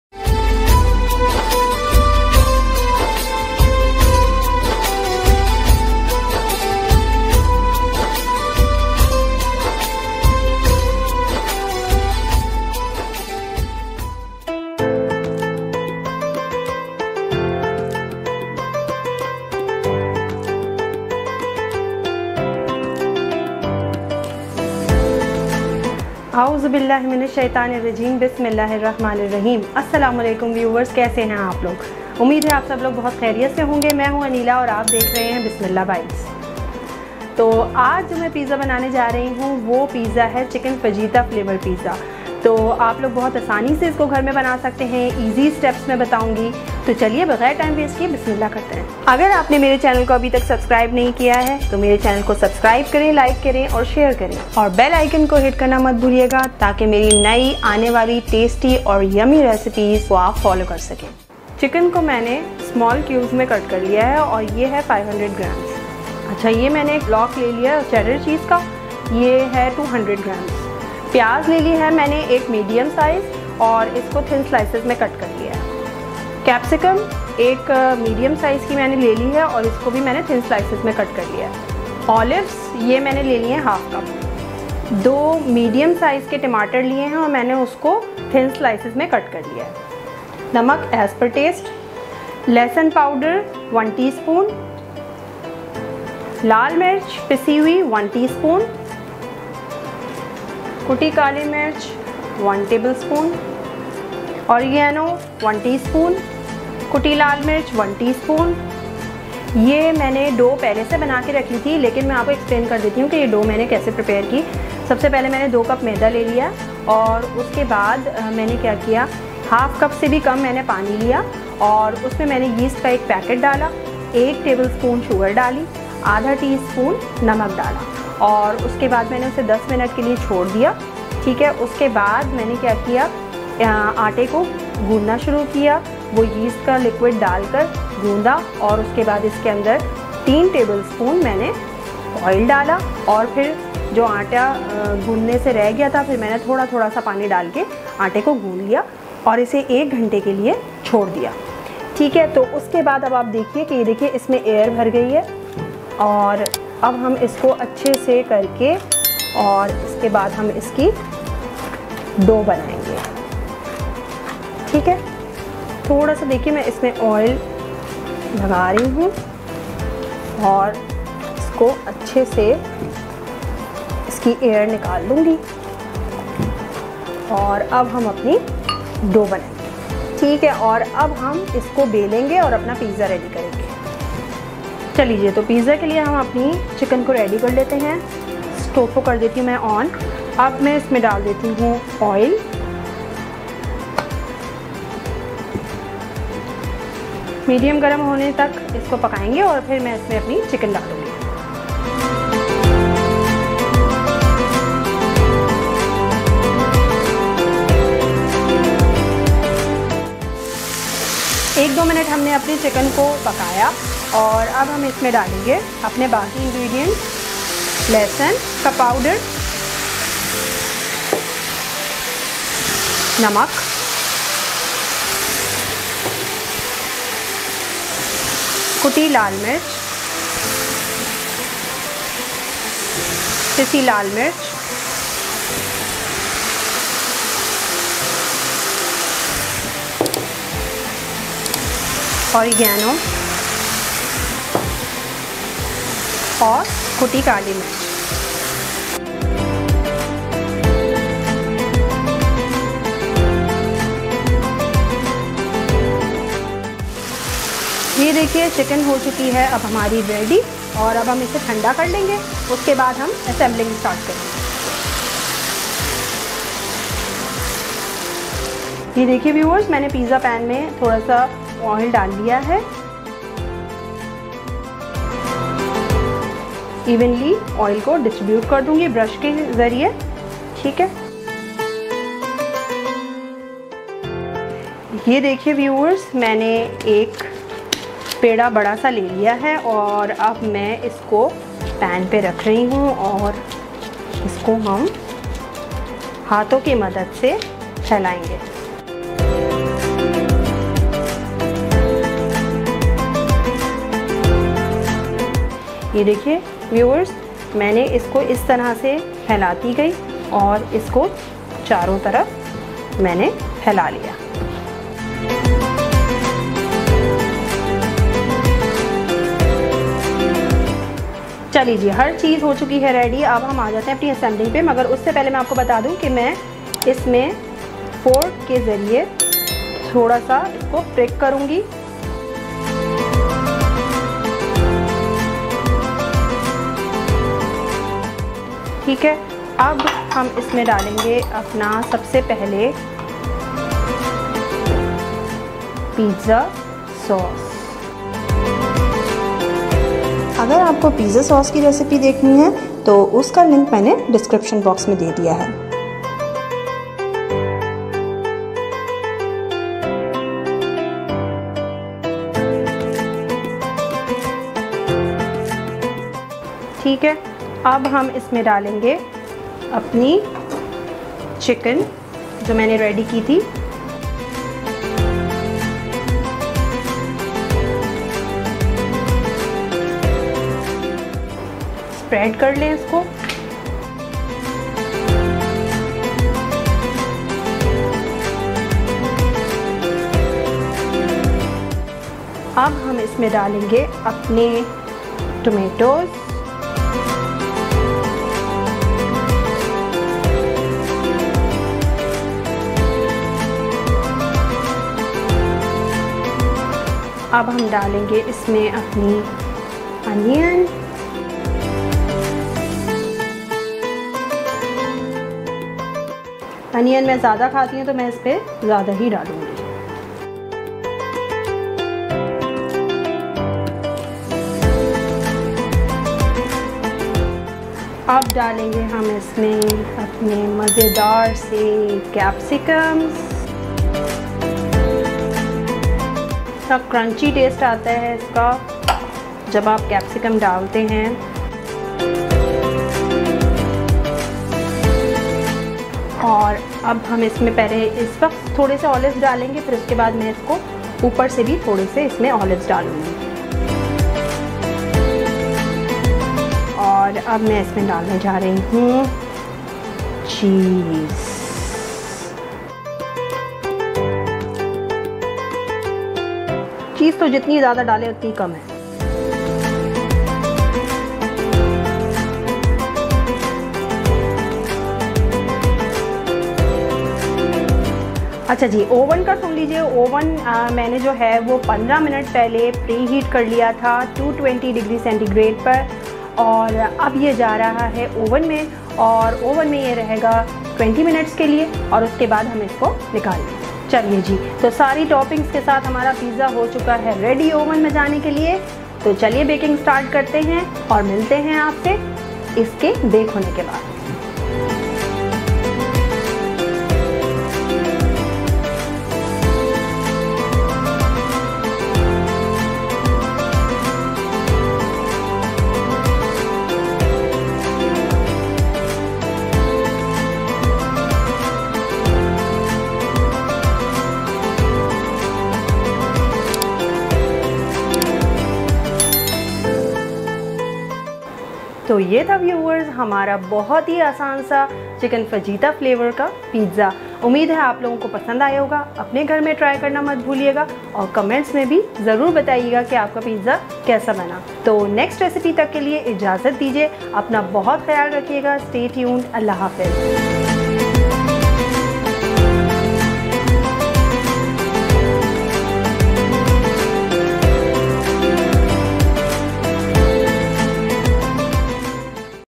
Easy Chicken Fajita Pizza Recipe How To Make Pizza At Home By Bismillah Bites (Ramzan Special)
Title: Easy Chicken Fajita Pizza Recipe How To Make Pizza At Home By Bismillah Bites (Ramzan Special)

Welcome to the channel "BISMILLAH BITES" where you will experience super delicious and mouth watering recipes along with some tips...

Easy and quick Chicken fajita Pizza Recipe Home Pizza Recipe. !